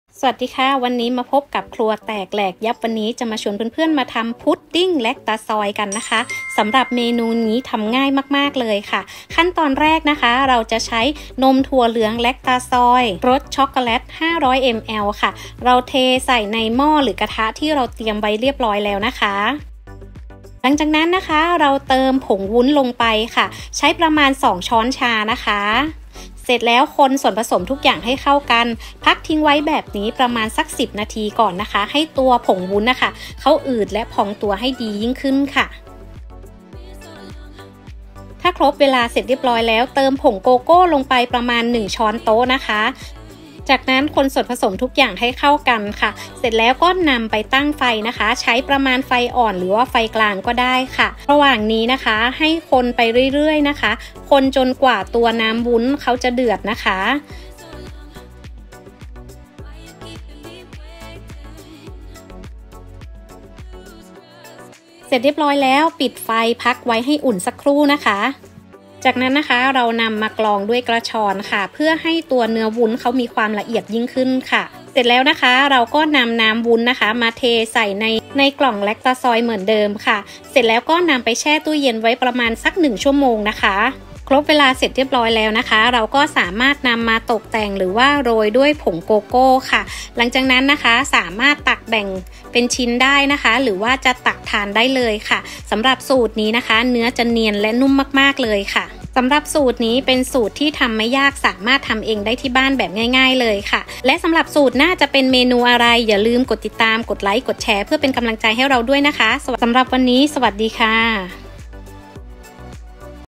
ใช้วัตถุดิบแค่ 3 อย่าง‼️ ทำ " พุดดิ้งแลคตาซอย "ทำง่าย ไม่ใช้เจลาติน |ครัวแตก แหลกยับ
นมถั่วเหลืองแลคตาซอย 500 มล.
ผงวุ้น 2 ชช.
ผงโกโก้ 1 ชต.
ผงโกโก้ (ใช้ตกแต่ง)